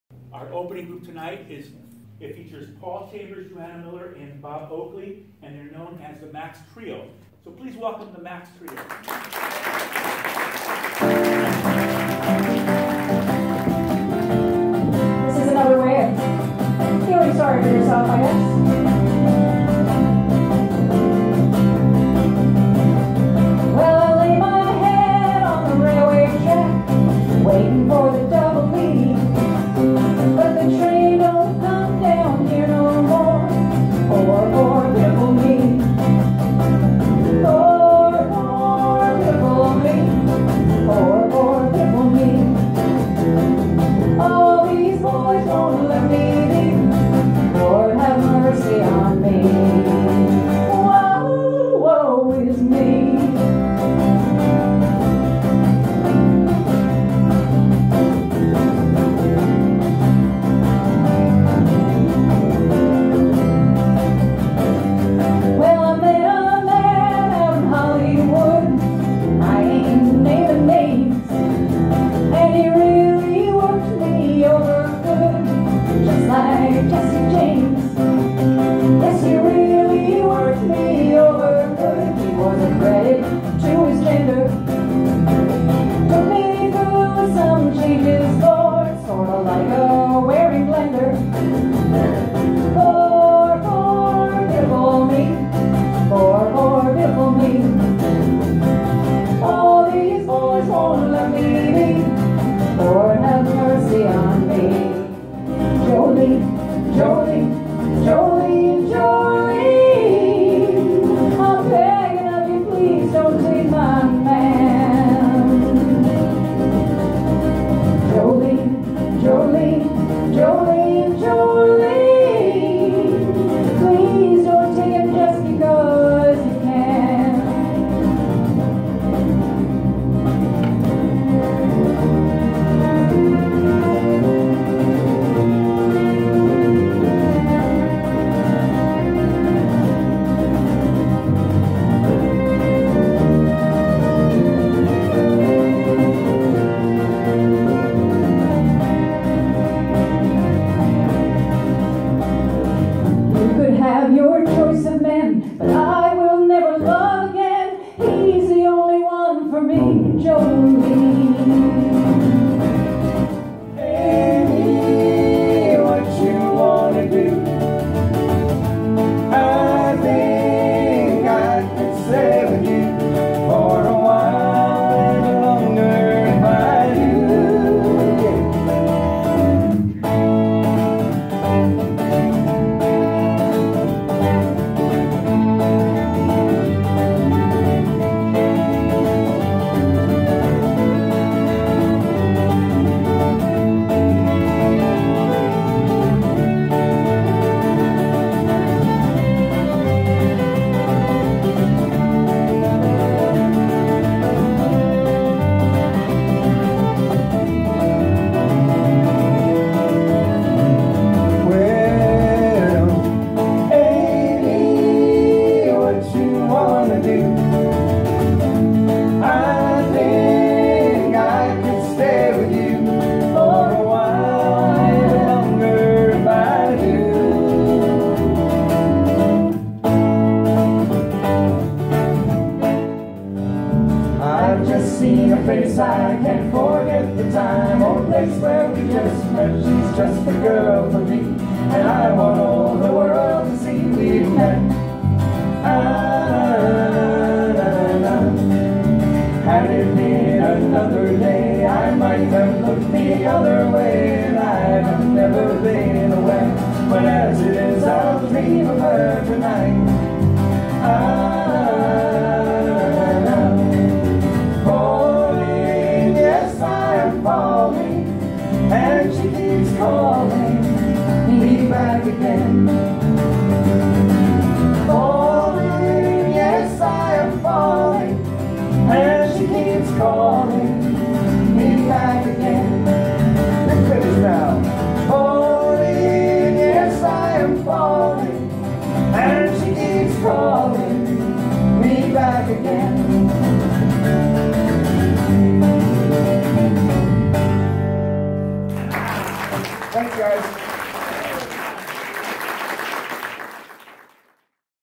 The Macks acoustic trio - Live on Stage
A selection of tunes recently performed live by the acoustic trio of The Macks.